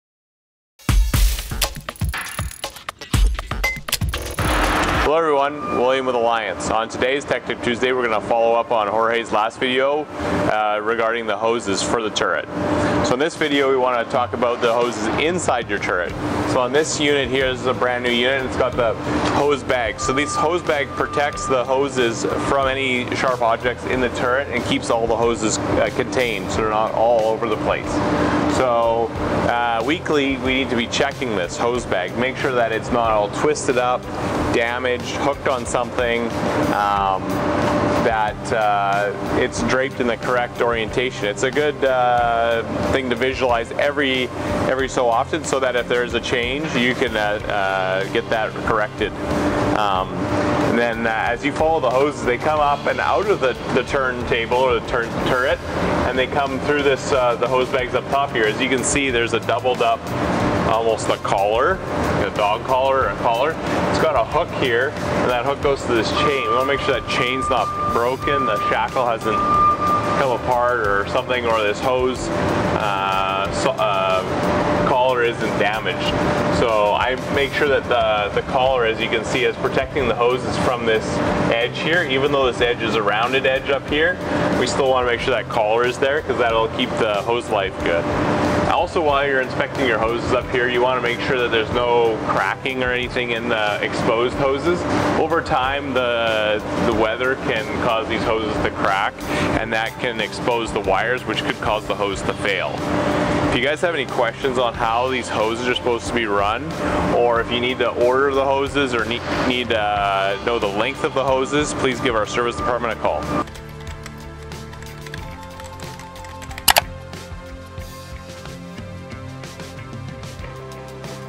Hose Bag
This week William reviews hose maintenance on your boom pump.